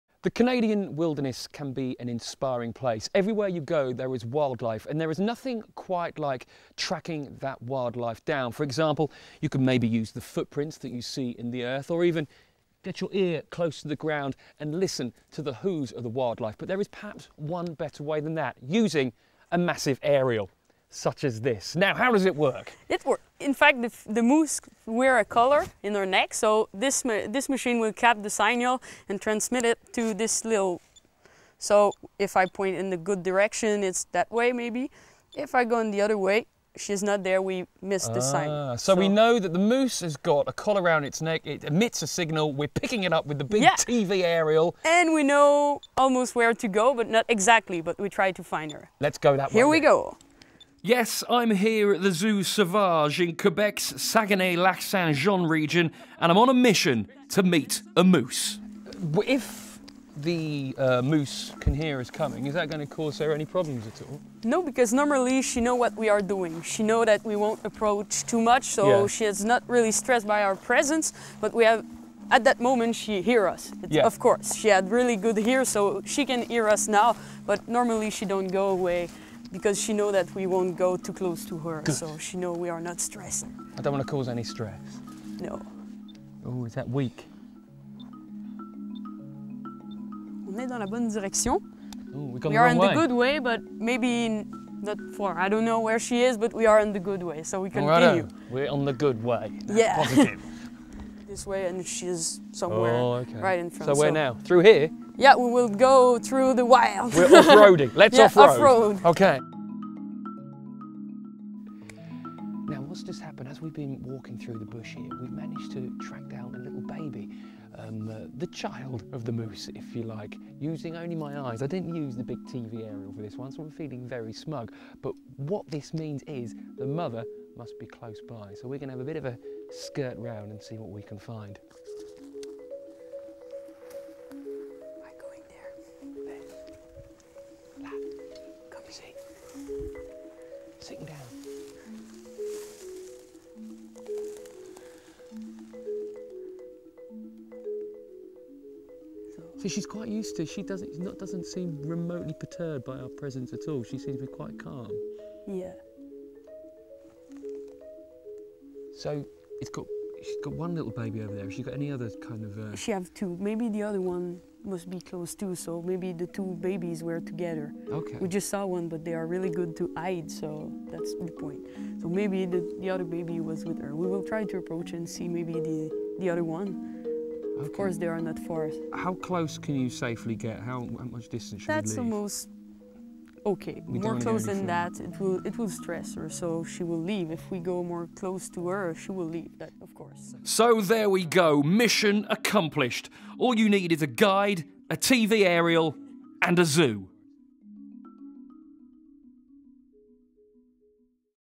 How to track a moose - filmed in Quebec, Canada
How to track a moose - filmed at the Zoo Sauvage, in Quebec, Canada